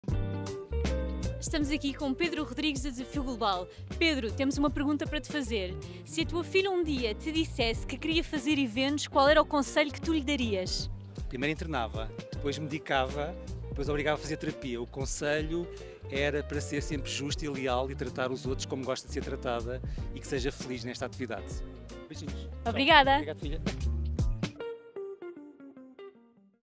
Reinvent the Event - Sabe o que é preciso para trabalhar em eventos?
Durante o Reinvent the Event - Endless Evolution, Pedro Rodrigues, director-geral da Desafio Global, deixou alguns conselhos a quem quer seguir uma profissão ligada aos eventos.

By Padrão